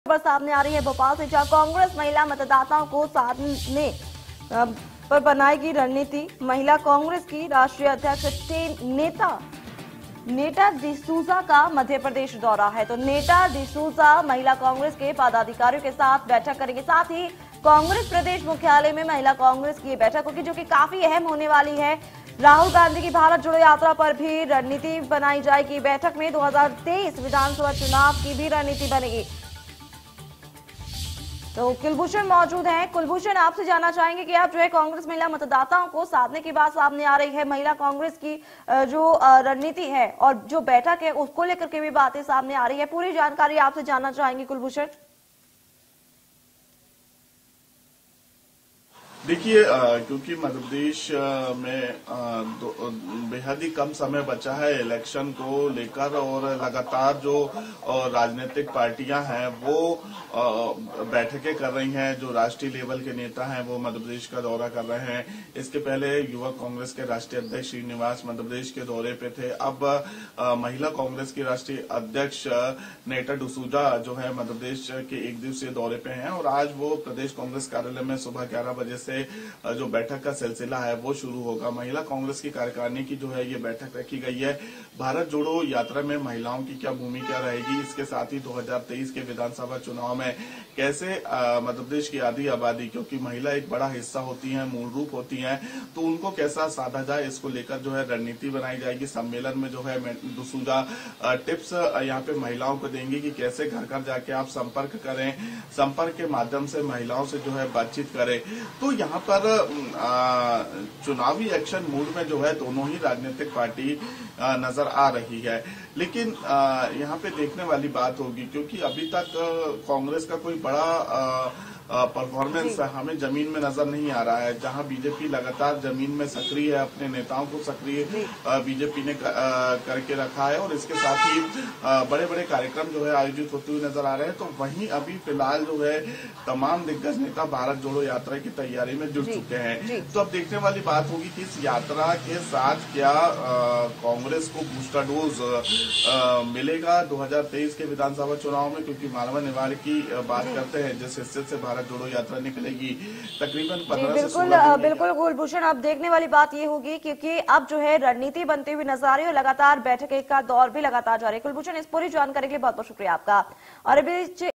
Bhopal News : कांग्रेस महिला मतदाताओं को साधने की बनेगी रणनीति
Bhopal News : कांग्रेस महिला मतदाताओं को साधने की बनेगी रणनीति,महिला कांग्रेस की राष्ट्रीय अध्यक्ष नेटा डिसूजा का मध्यप्रदेश दौरा

Bhopal News : Congress will make a strategy to cater to women voters, National President of Mahila Congress Neta D'Souza visits Madhya Pradesh